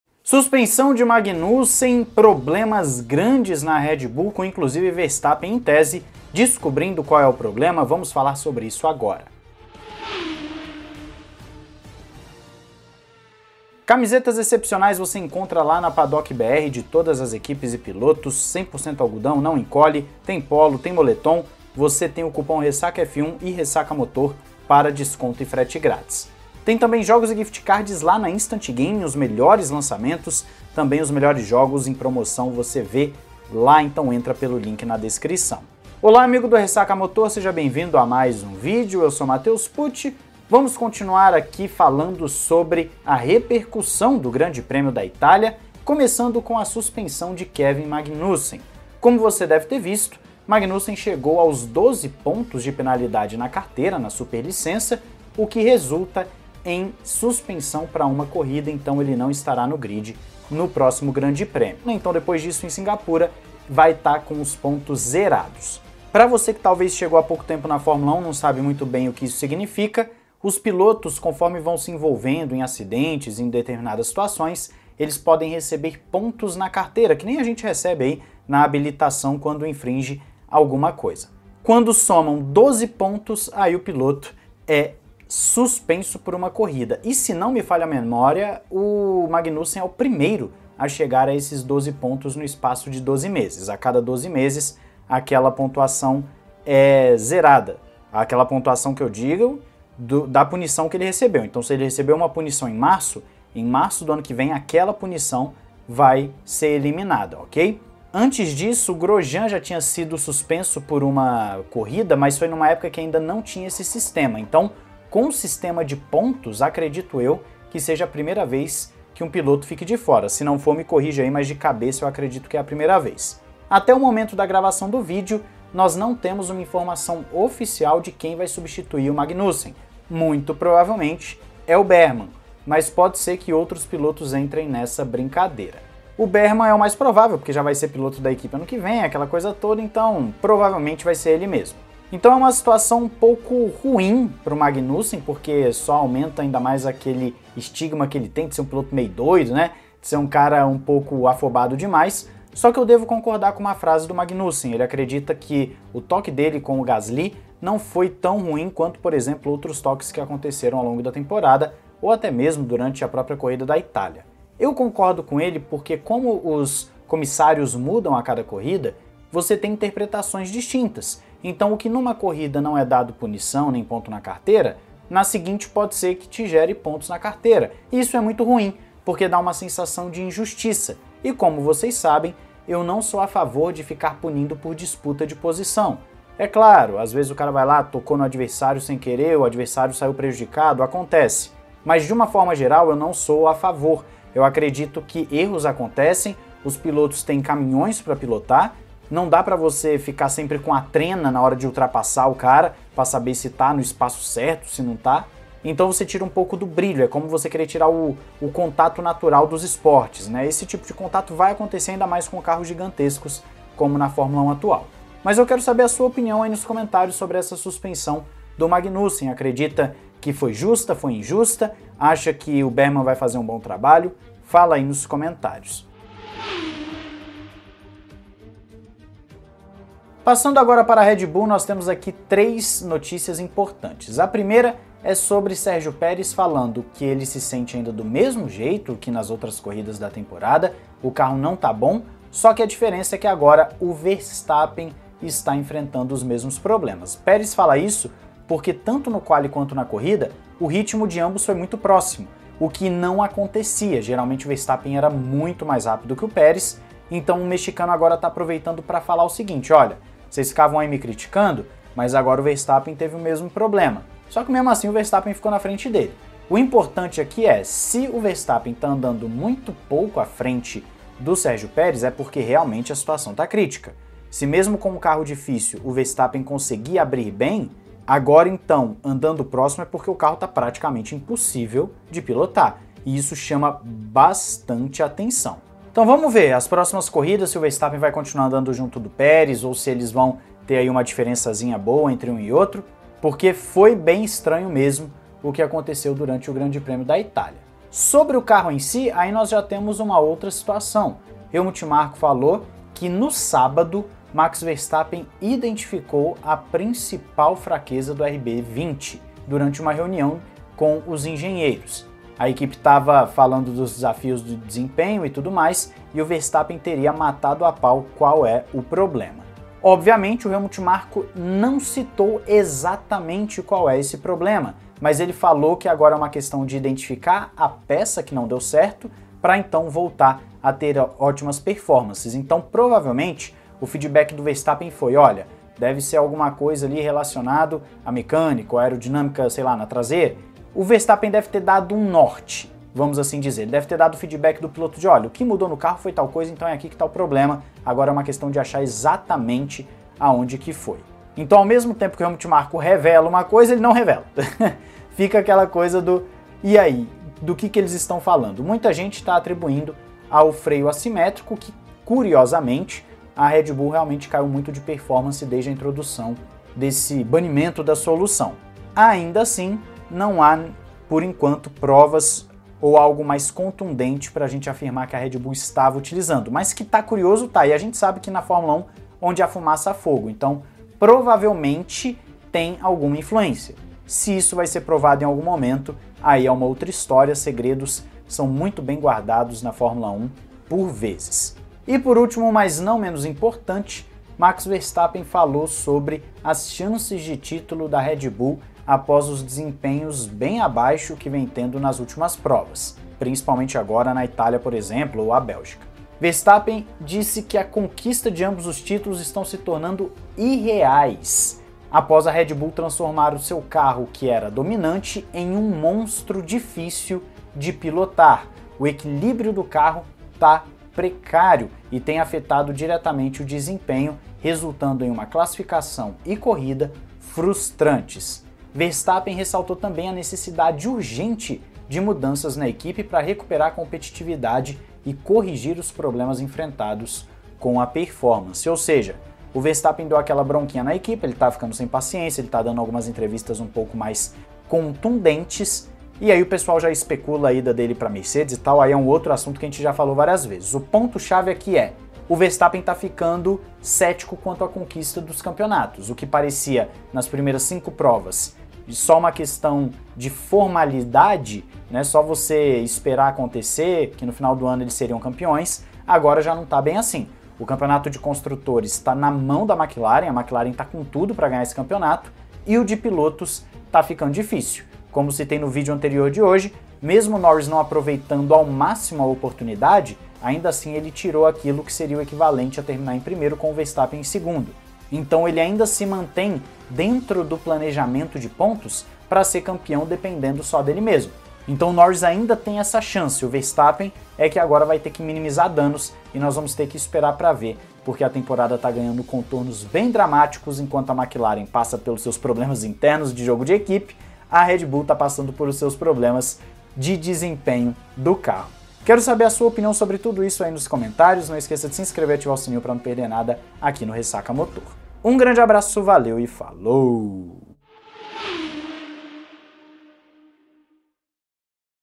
MAX CHAMA RB20 DE MONSTRO INGUIÁVEL E NÃO CRÊ NA CONQUISTA DO TÍTULO / PEREZ / MAGNUSSEN
Sergio Milani
@scuderiamilani (Twitter)

João 3:16

00:00 - Introdução
00:46 - Suspensão de Magnussen
02:34 - Inconsistencia nas punições
03:50 - Checo e a dificuldade de Max
05:00 - Max identificou o problema do RB20
06:34 - Max não acredita em ambos os títulos